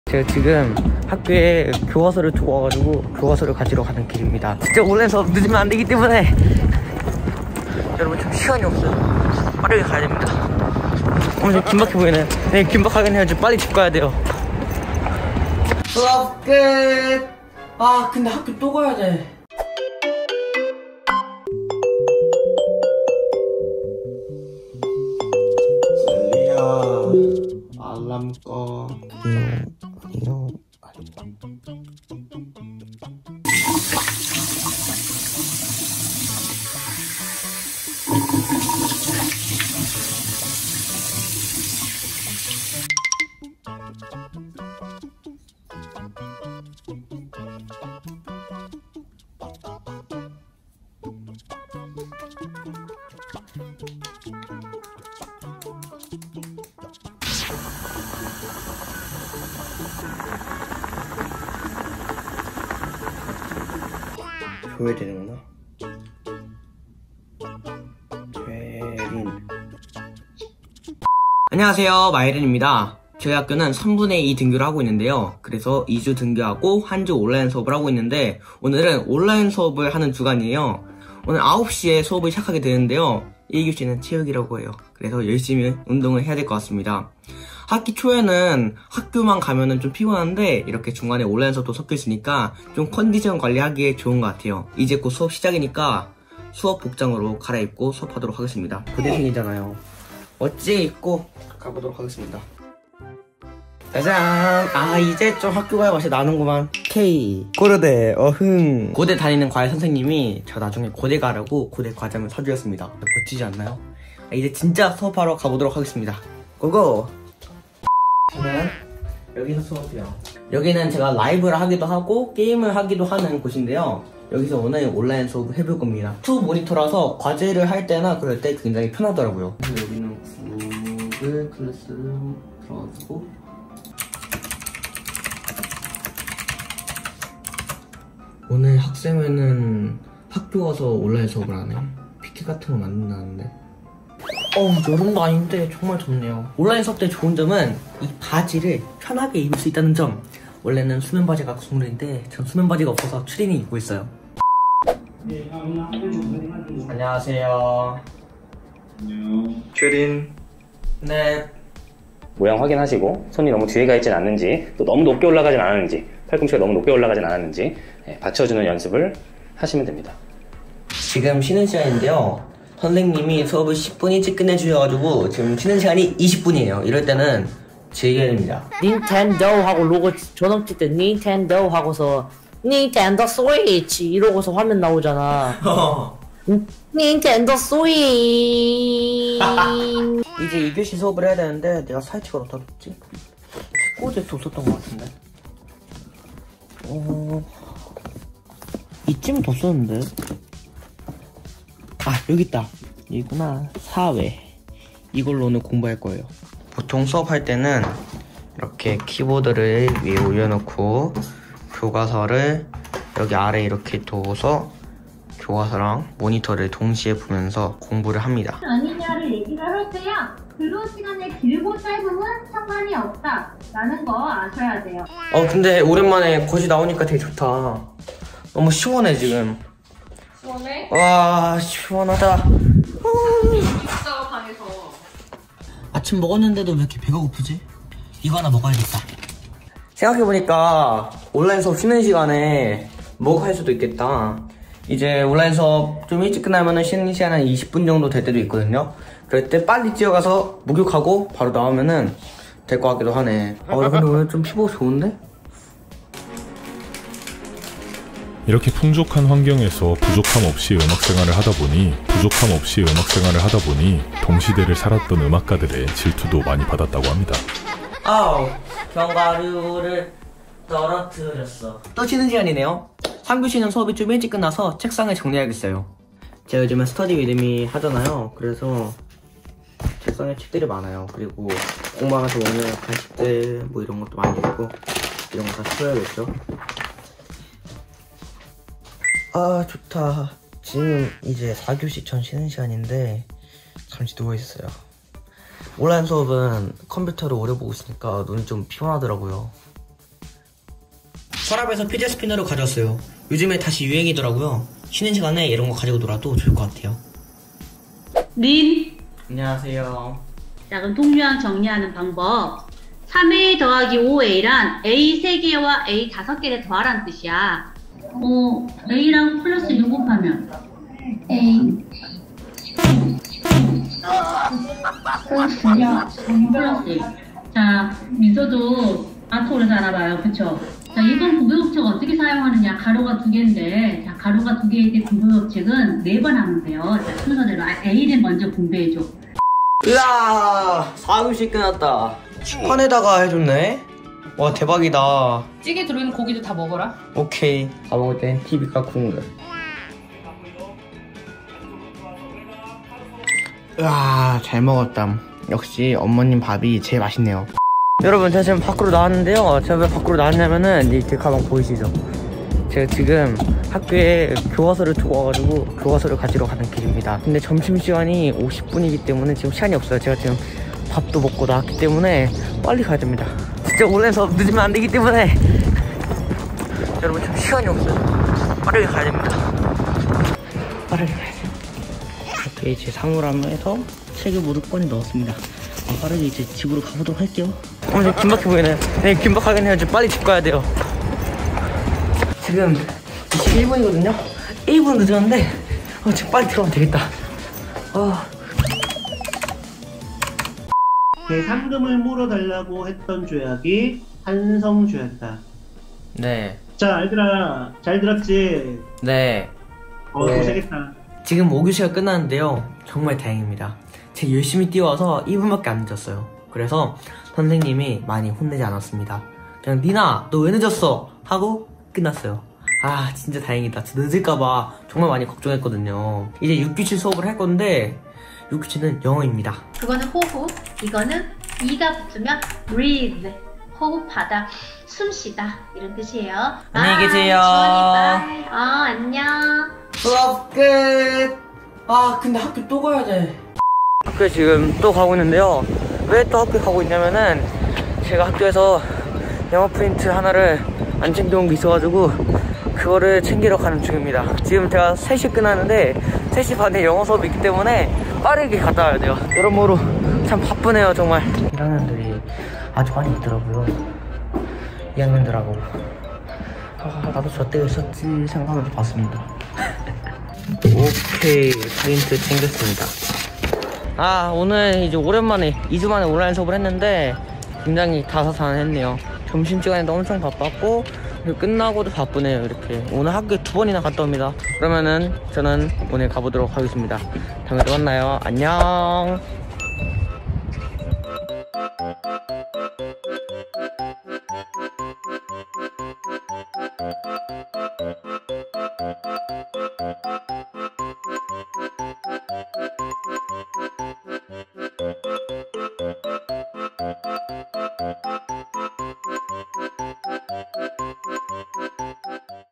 중학교 3학년 마지막 온라인 수업 일상 (쉬는시간, 점심시간에는 무엇을 했을까) | 마이린 TV
마이린 2주만에 다시 온라인 수업을 하게 되었어요
마이린 학교는 2/3 등교를 하는데, 금주는 온라인 수업!
다음주부터는 이제 온라인 수업을 하고 모두 등교를 한다고 해서
금주가 마지막 온라인 수업 주간이에요

온라인 수업 쉬는 시간, 점심시간, 그리고 종례 후에는 여러분 무엇을 하나요?
오늘 마이린 아침 기상부터 쉬는시간, 점심시간, 종례 후 일상을 여러분과 함께 공유해요. 여러분의 일상과 어떤 점이 다른지, 한번 봐주세요~